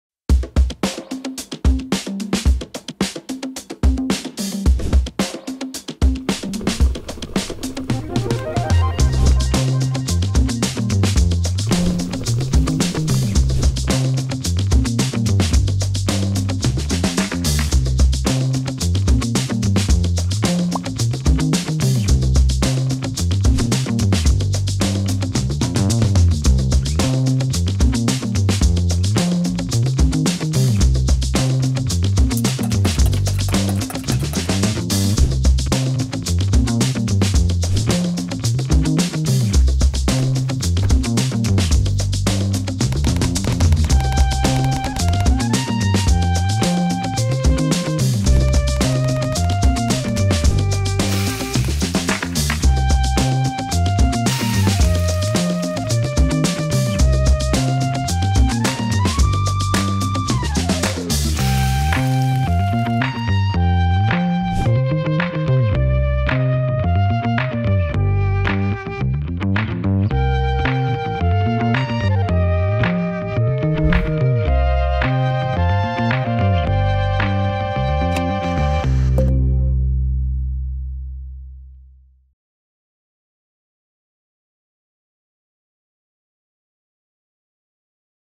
How to make schupfnudeln (finger noodles) with garlic butter | taste.com.au
Drenched with melted garlic butter, these little potato "finger noodles" (also known as "fingernudel"), make the ultimate snack, side dish or finger food. They're super easy to make: simply mix the dough, roll by hand into long dumplings, then fry in a pan until golden.